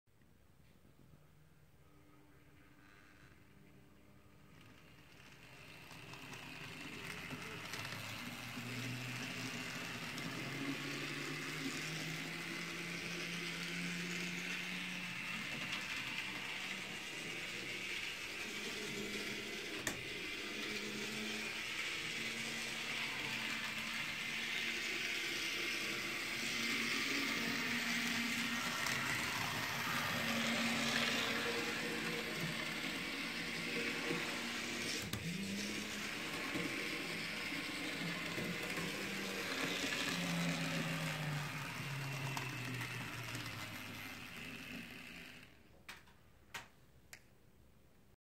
NVJ-712-35285
Dapol N Gauge B17 been tested after repair in DC